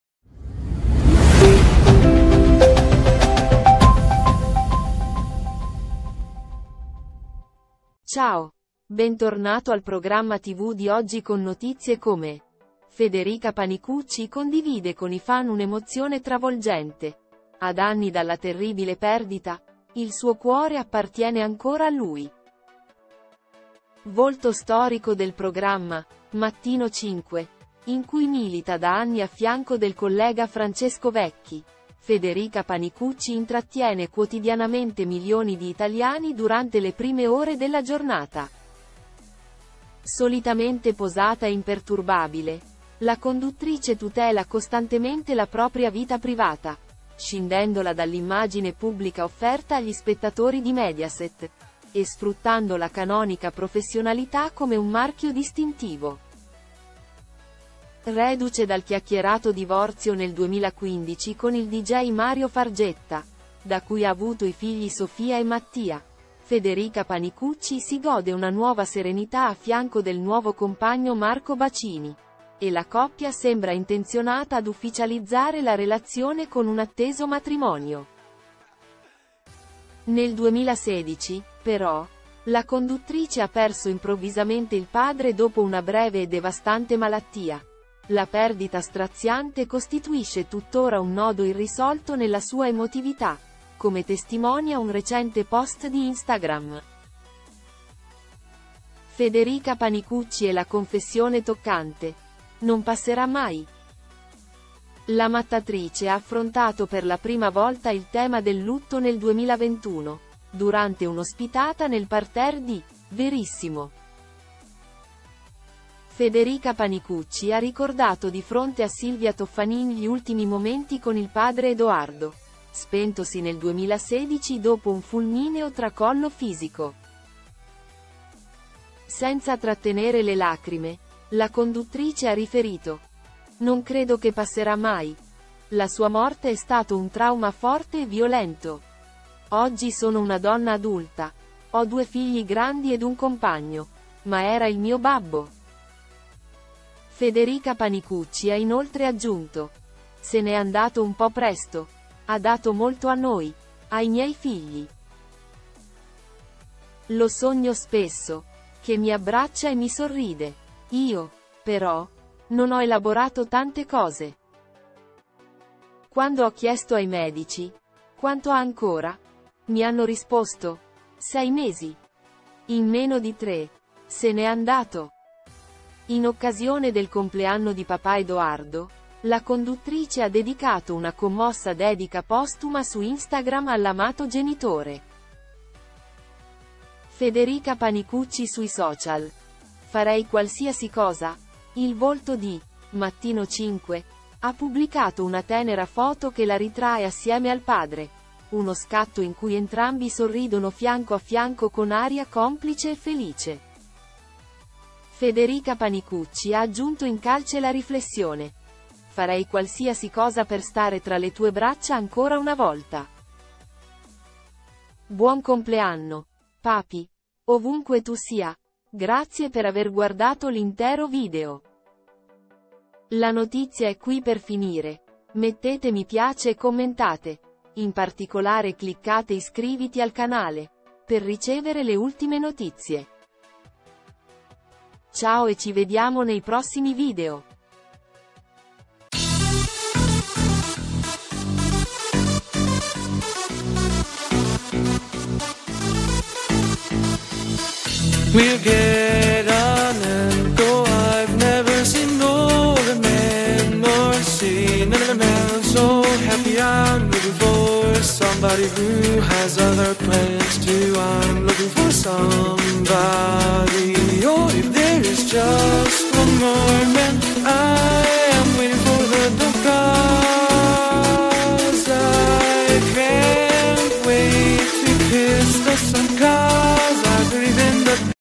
Federica Panicucci con il cuore in gola il tristissimo messaggio Purtroppo non c’è più
Volto storico del programma “Mattino 5”, in cui milita da anni a fianco del collega Francesco Vecchi, Federica Panicucci intrattiene quotidianamente milioni di italiani durante le prime ore della giornata.

Solitamente posata e imperturbabile, la conduttrice tutela costantemente la propria vita privata, scindendola dall’immagine pubblica offerta agli spettatori di Mediaset, e sfruttando la canonica professionalità come un marchio distintivo.